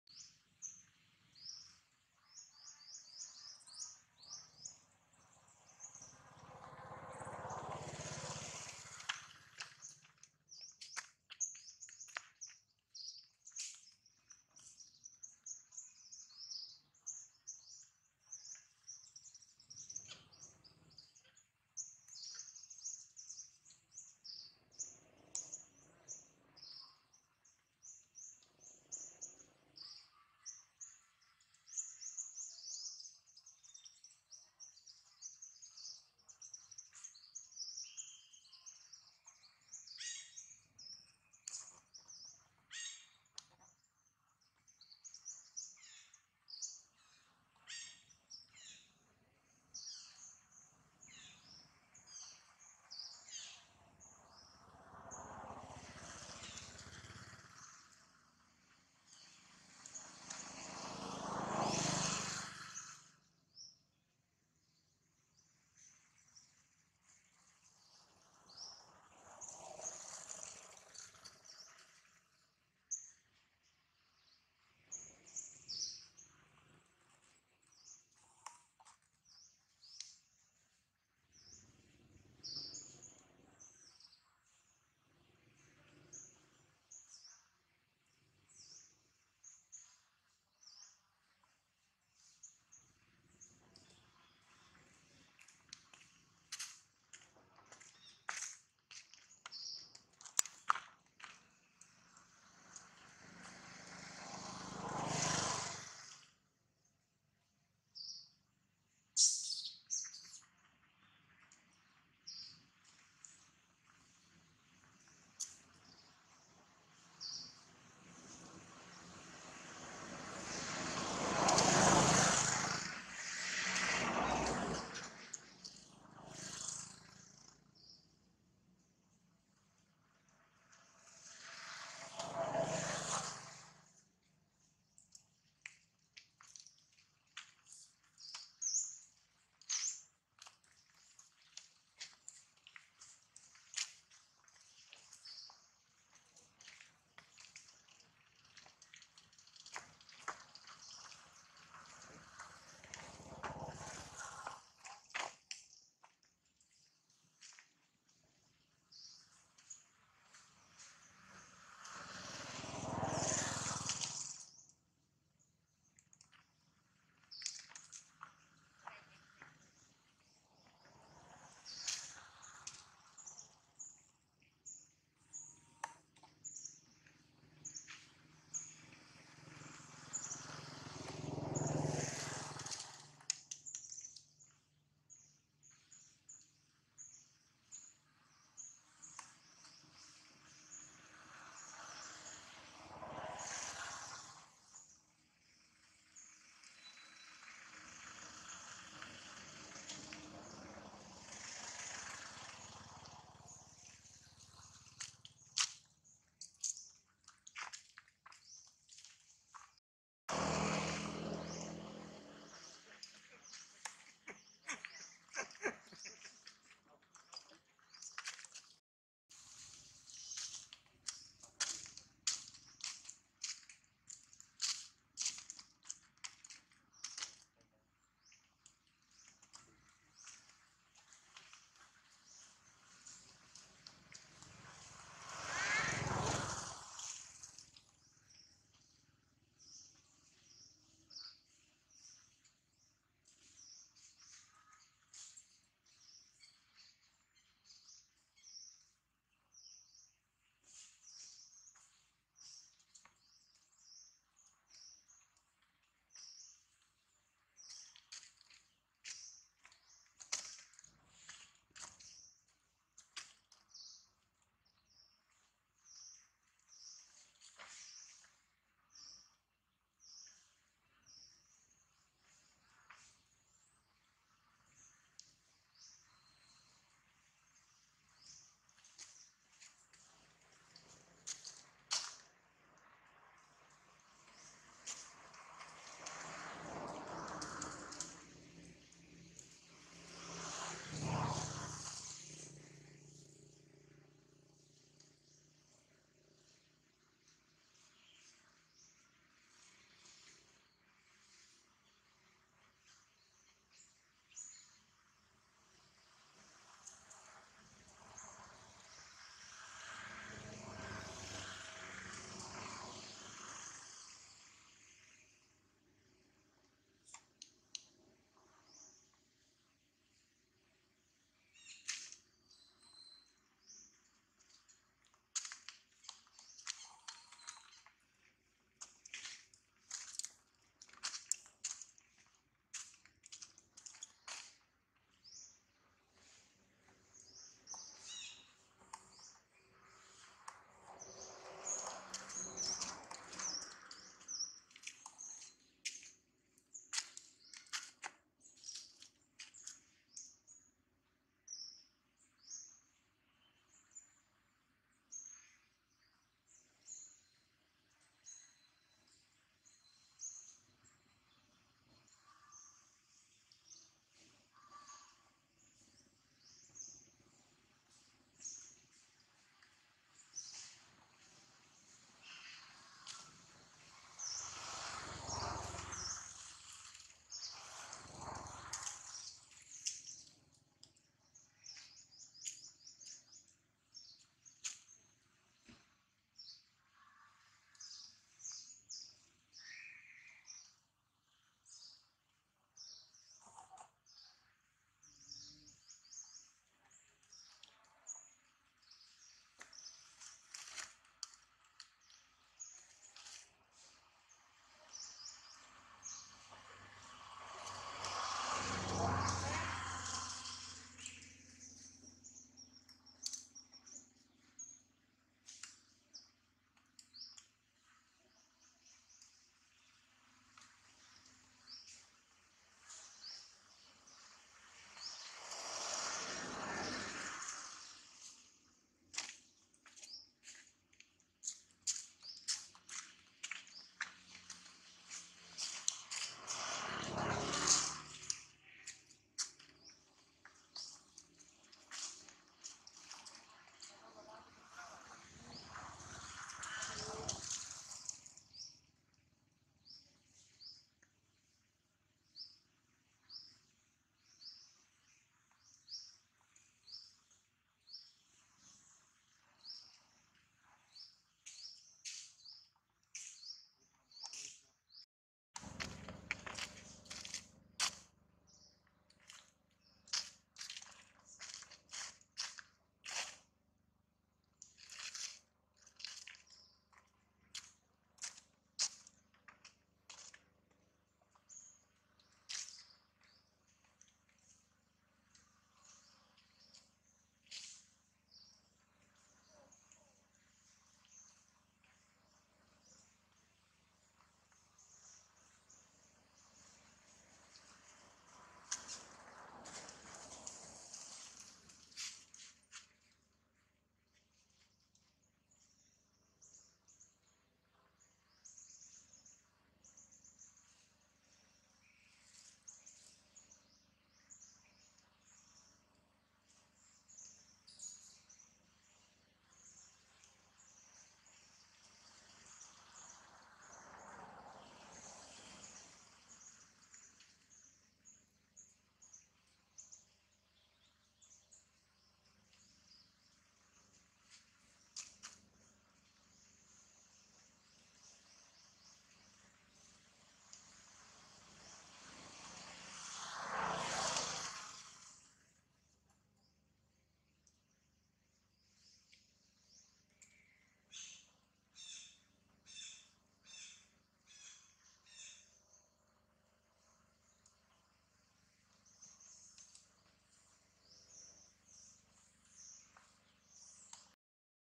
MY GOD ! Poor baby Jane come to beg mom Jane for hug & milk before Jane will giving born soon
Everyday Monkey